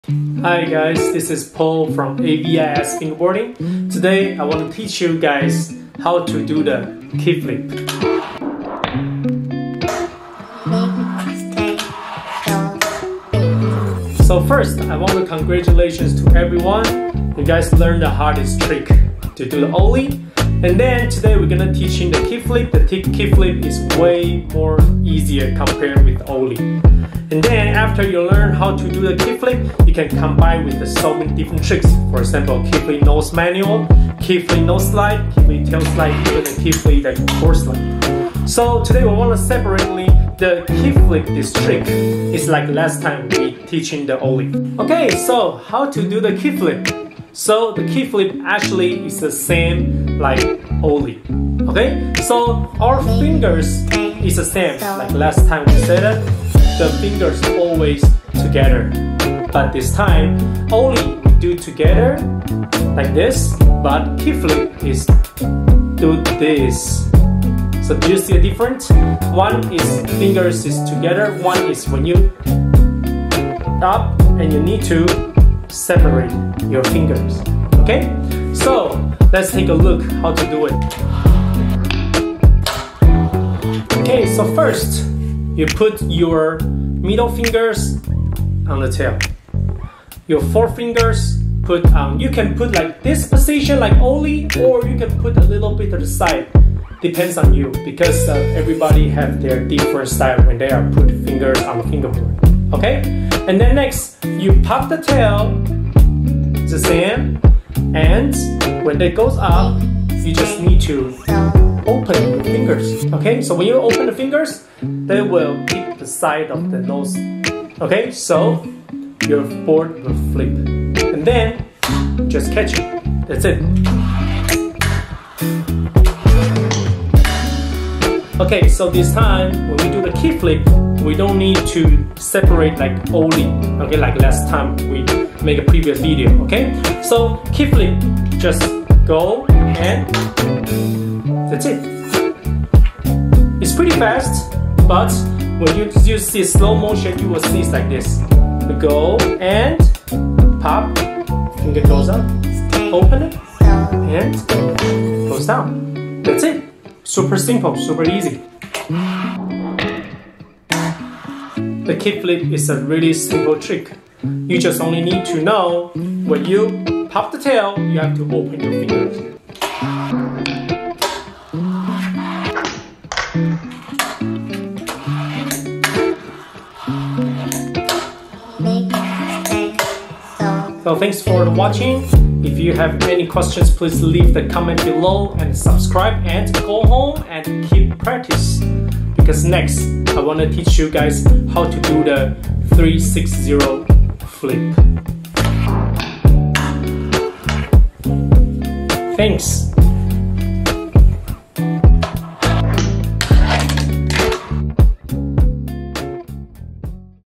AVIS FINGERBOARD TUTORIAL - How To Do The Kick Flip
Hi guys this is Po from AVIS Fingerboarding, Let's do the kick flip today, if you like it please leave the comment below share with me how did you practice kick flip and subscribe and keep practicing!